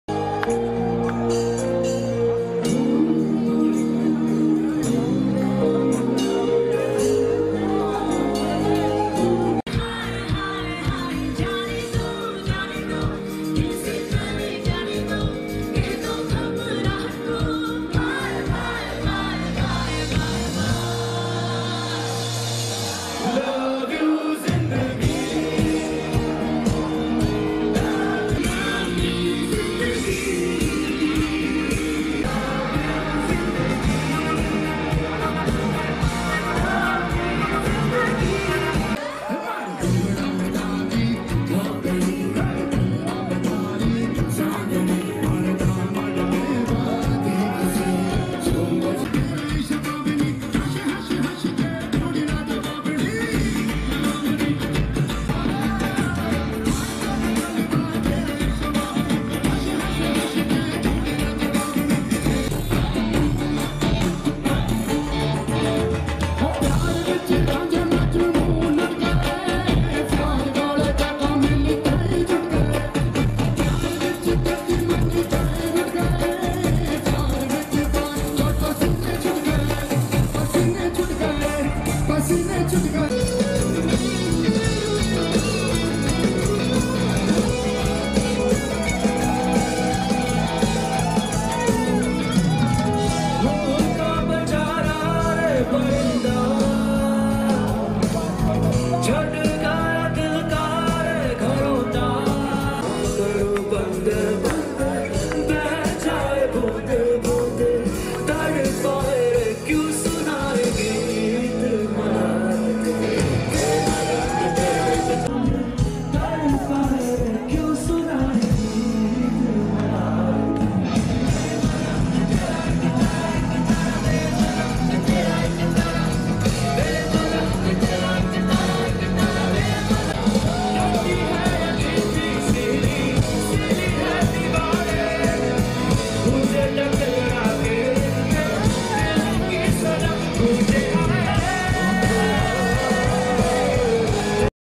कानपुर : अमित त्रिवेदी के गानों के साथ संपन्न हुआ IIT का अंतराग्नि फेस्टिवल
कानपुर : आईआईटी में चल रहा  53वां अंतराग्नि फेस्टिवल गायक अमित त्रिवेदी के कार्यक्रम के साथ समाप्त हो गया. चार दिन चले इस फेस्टिवल में एक से बढ़कर एक कार्यक्रम हुए. बॉलीवुड के गानों और डीजे की धुन पर शिरकत करने पहुंचे देश भर के होनहार छात्र-छात्राएं मस्ती में डूबे दिखाई दिए.